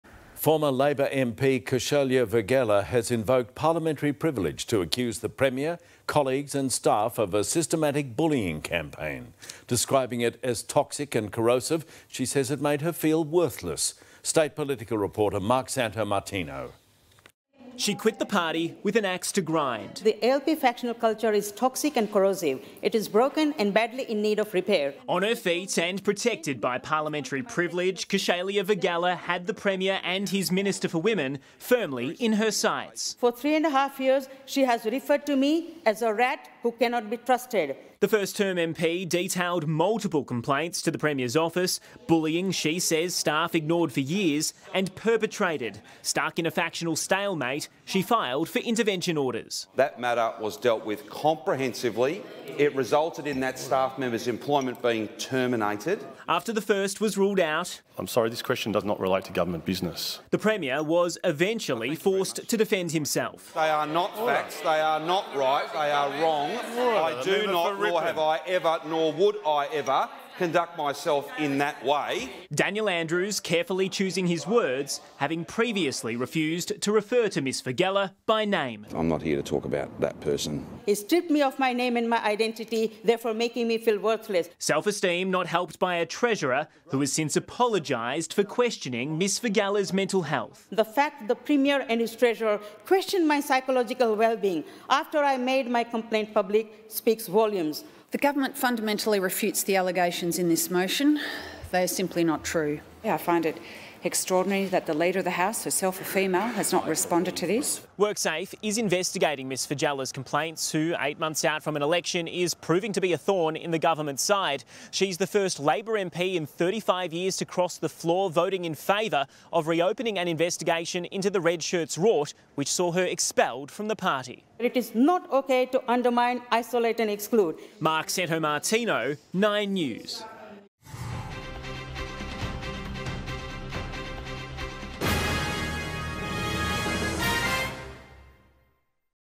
Victorian Premier and staff accused of 'systematic bullying' | 9 News Australia
Former Labor MP turned crossbencher Kaushaylia Vaghela has invoked parliamentary privilege, accusing Victorian Premier Daniel Andrews and other Labor members and staff for embarking in a “systematic bullying campaign” that she says continued for years.